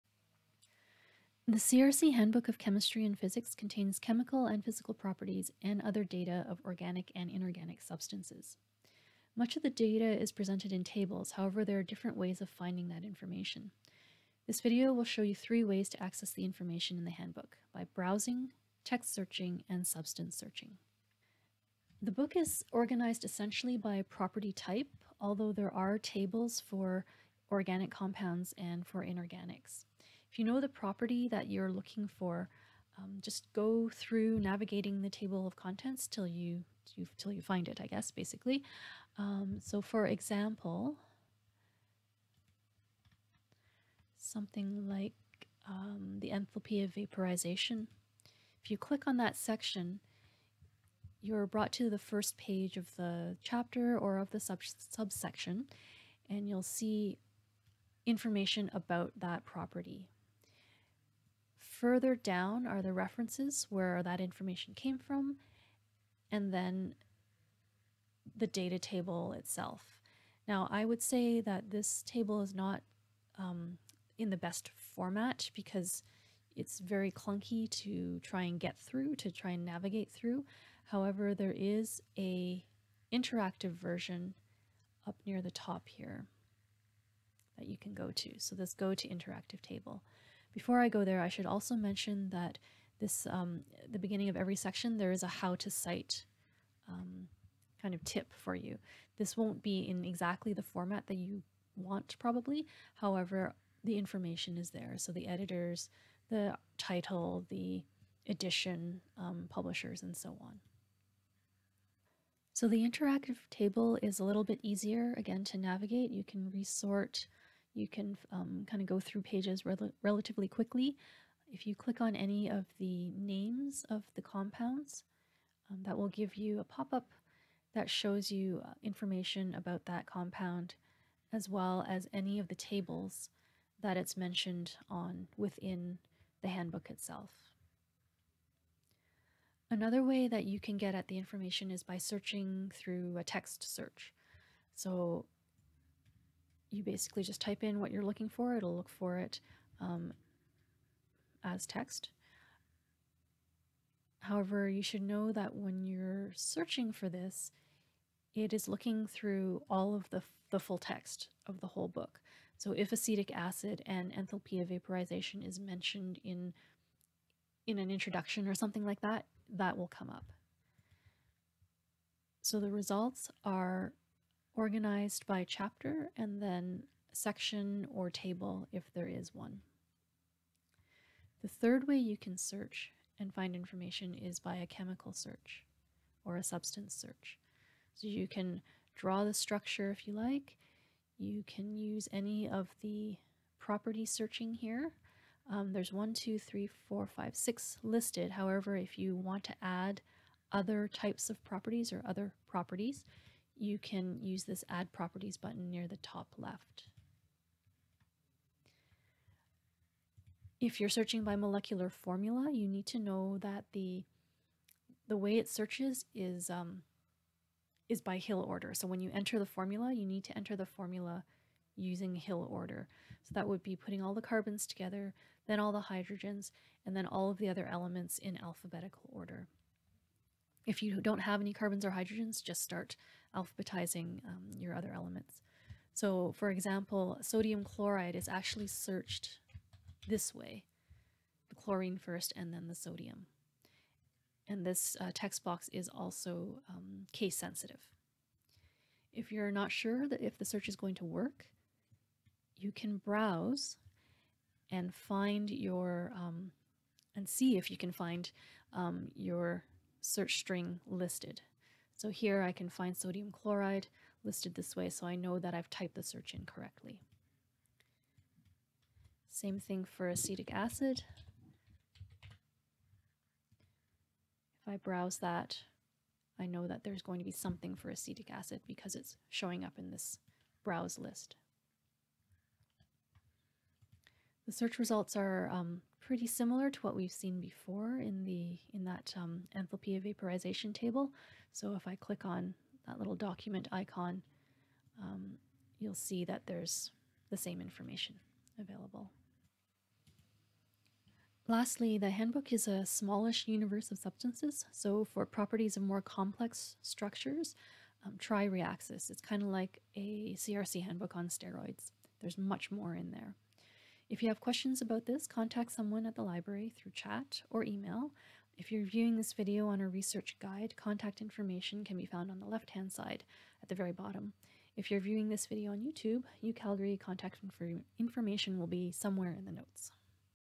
CRC Handbook of Chemistry and Physics
The CRC Handbook is a good source of physical and chemical properties.
This video shows three ways to find information in the Handbook.

Access the CRC Handbook (UCalgary students, faculty, and staff only!!) via the library's CHEM371 guide